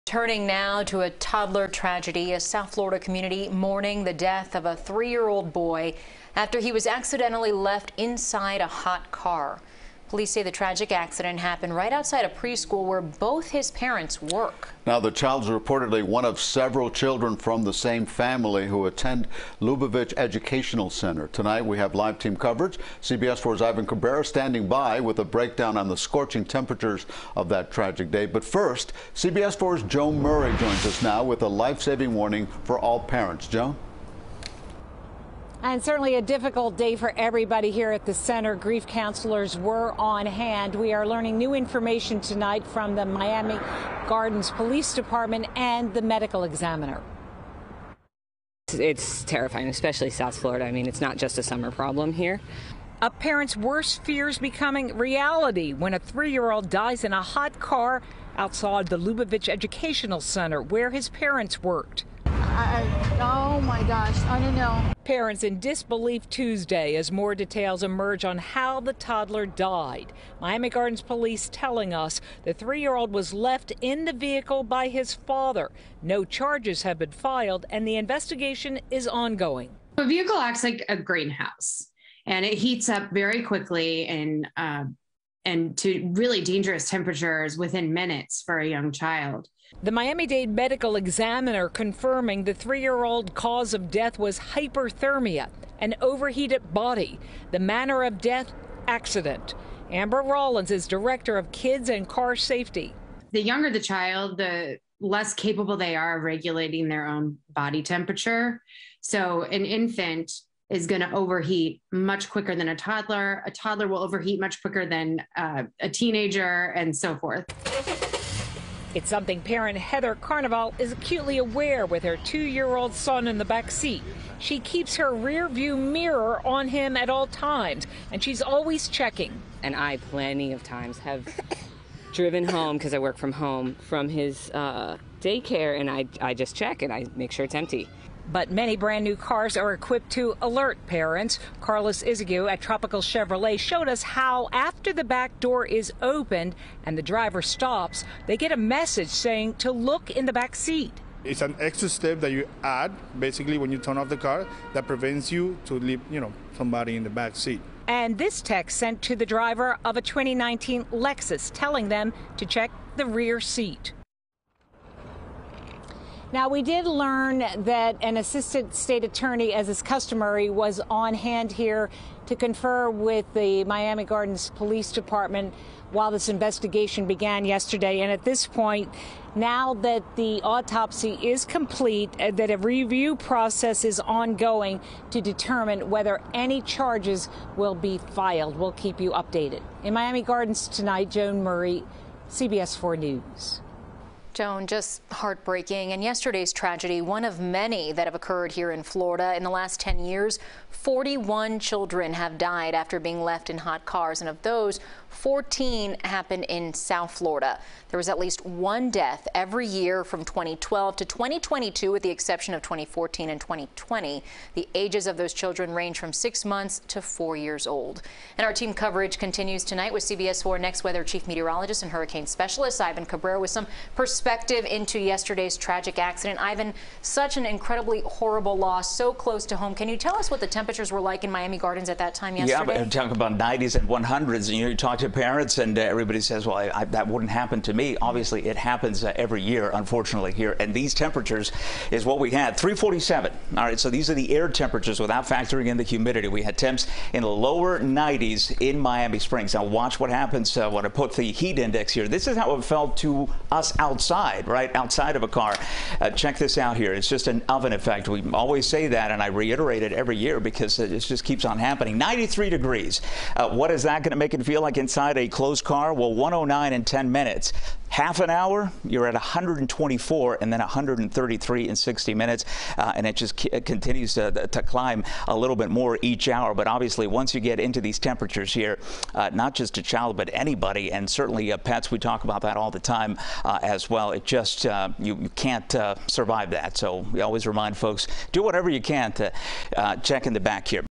UPDATE: 3-Year-Old Dies After Being Left In A Hot Car In Miami Gardens
The tragic incident happened in the parking lot of an elementary school in Miami Gardens.